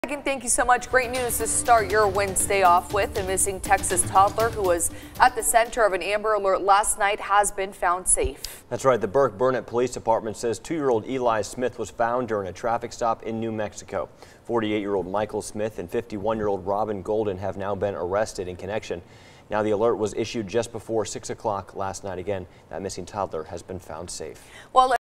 UPDATE: Missing Burkburnett toddler found safe
The toddler was found safe in New Mexico.